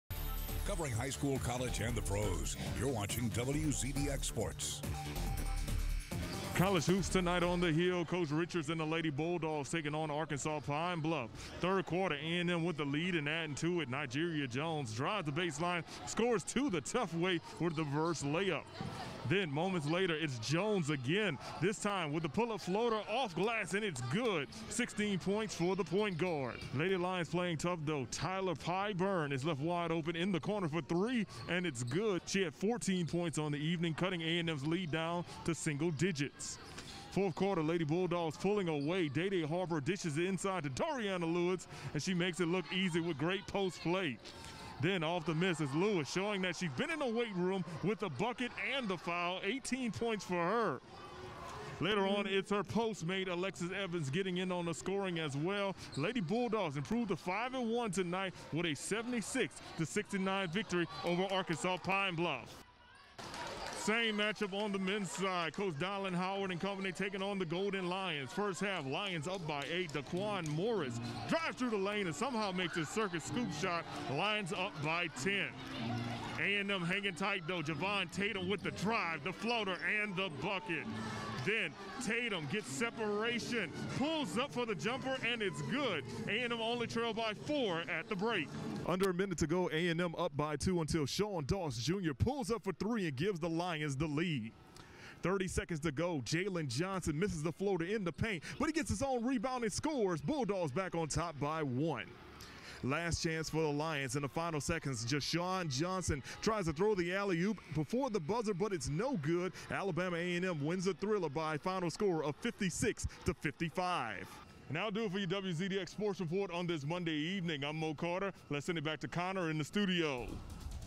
Bulldogs & Lady Bulldogs pick up home victories against UAPB (Highlights)
Dariauna Lewis had a double-double of 18 points and 12 boards in the Lady Bulldogs 76-69 win. Jalen Johnson's putback basket lifted AAMU to a 56-55 win for the men.